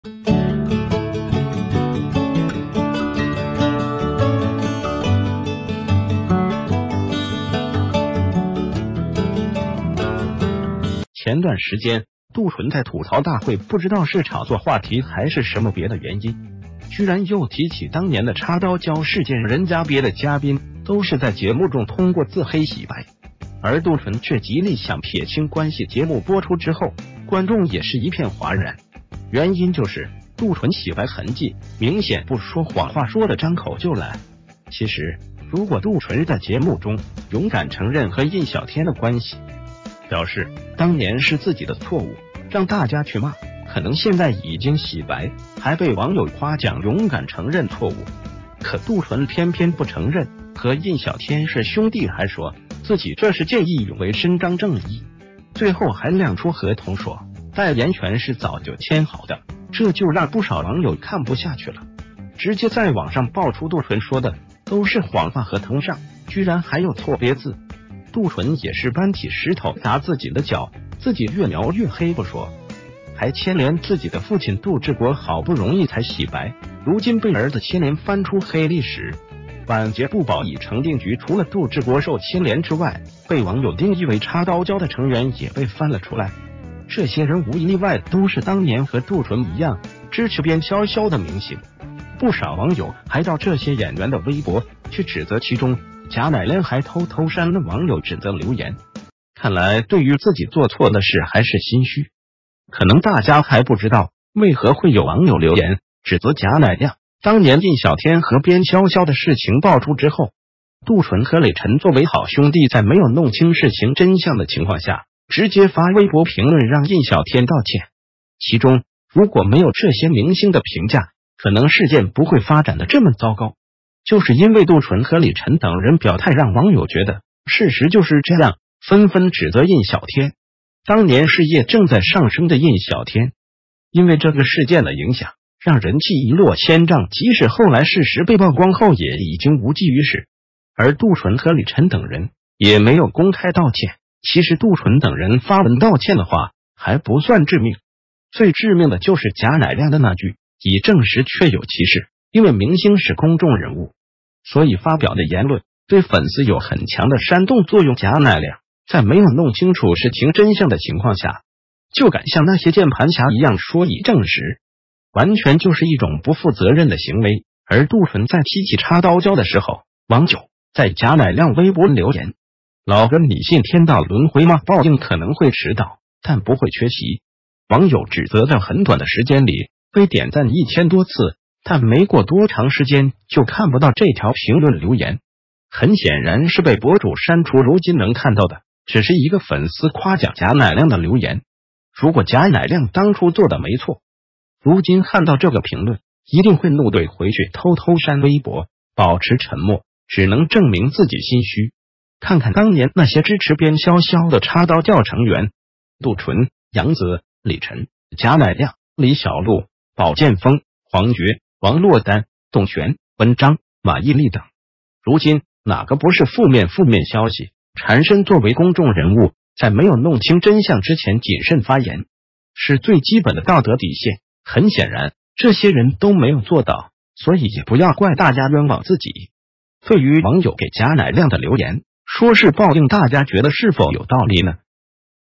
賈乃亮刪網友留言證明心虛，當年一句“已證實”害慘印小天
谢谢！

    前段時間，杜淳在《吐槽大會》不知道是炒作話題，還是什么別的原因，居然又提起當年的插刀教事件，人家別的嘉賓都是在節目種通過自黑洗白，而杜淳卻極力想撇清關系，節目播出之后觀眾也是一片嘩然，原因就是杜淳洗白痕跡明顯不說，謊話說的張口就來。
    其實如果杜淳在節目中勇敢承認和印小天的關系，表示當年是自己的錯誤讓大家去罵，可能現在已經洗白還被網友夸獎勇敢承認錯誤，可杜淳偏偏不承認和印小天是兄弟，還說自己這是見義勇為伸張正義，最后還亮出合同說代言權是早就簽好的。
    這就讓不少網友看不下去了，直接在網上爆出杜淳說的都是謊話，合同上居然還有錯別字，杜淳也是搬起石頭砸自己的腳，自己越描越黑不說，還牽連自己的父親杜志國，好不容易才洗白，如今被兒子牽連翻出黑歷史，晚節不保已成定局。
    除了杜志國受牽連之外，被網友定義為插刀教的成員也被翻了出來，這些人無一例外都是當年和杜淳一樣支持邊瀟瀟的明星，不少網友還到這些演員的微博去指責，其中賈乃亮還偷偷刪了網友指責留言，看來對于自己做錯的事還是心虛。
    可能大家還不知道為何會有網友留言指責賈乃亮，當年印小天和邊瀟瀟的事情爆出之后，杜淳和李晨作為好兄弟在沒有弄清事情真相的情況下，直接發微博評論讓印小天道歉，其中如果沒有這些明星的評價，可能事件不會發展的這么糟糕，就是因為杜淳和李晨等人表態，讓網友覺得事實就是這樣，紛紛指責印小天。
    當年事業正在上升的印小天，因為這個事件的影響讓人氣一落千丈，即使后來事實被曝光后也已經無濟于事，而杜淳和李晨等人也沒有公開道歉，其實杜淳等人發文道歉的話還不算致命，最致命的就是賈乃亮的那句“已證實，確有其事”。
    因為明星是公眾人物，所以發表的言論對粉絲有很強的煽動作用，賈乃亮在沒有弄清楚事情真相的情況下，就敢像那些鍵盤俠一樣說已證實，完全就是一種不負責任的行為，而杜淳再提起插刀教的時候，網友在賈乃亮微博留言：老哥你信天道輪回嗎？報應可能會遲到但不會缺席。
    網友指責在很短的時間里被點贊一千多次，但沒過多長時間就看不到這條評論留言，很顯然是被博主刪除，如今能看到的只是一個粉絲夸獎賈乃亮的留言，如果賈乃亮當初做的沒錯，如今看到這個評論一定會怒懟回去，偷偷刪微博保持沉默，只能證明自己心虛。
    看看當年那些支持邊瀟瀟的插刀教成員，杜淳、楊子、李晨、賈乃亮、李小璐、保劍鋒、黃覺、王珞丹、董璇、文章、馬伊琍等，如今哪個不是負面負面消息纏身，作為公眾人物在沒有弄清真相之前謹慎發言，是最基本的道德底線，很顯然這些人都沒有做到，所以也不要怪大家冤枉自己。
    對于網友給賈乃亮的留言說是報應，大家覺得是否有道理呢？